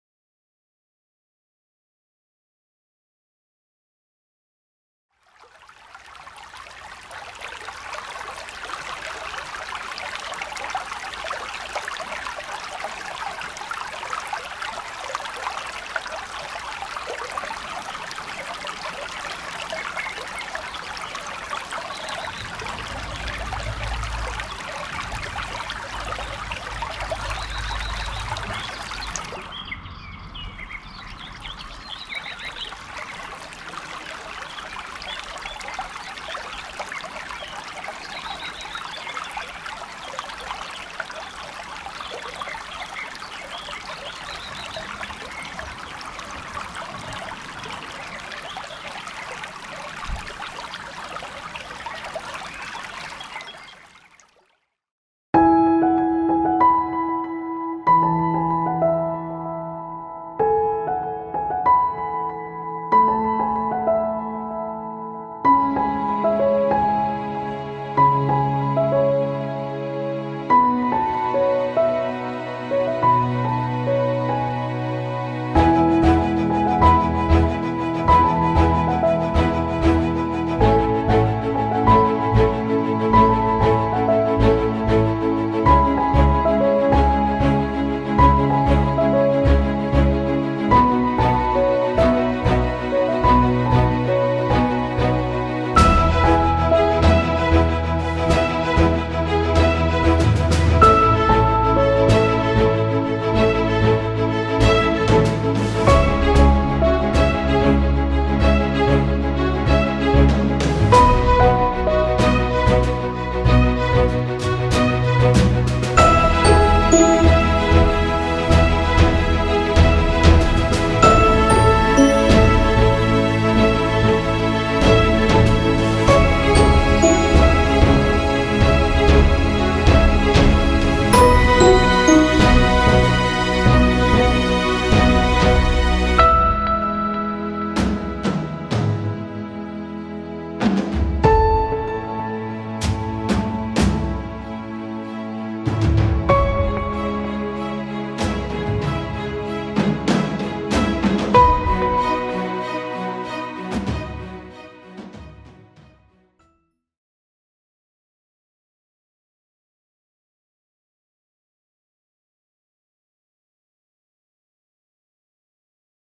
Devant L'Objectif présente le gîte Ardenne Céleste.
Visite virtuelle ou vidéo promotionnelle, Devant L'Objectif Booste votre visibilité.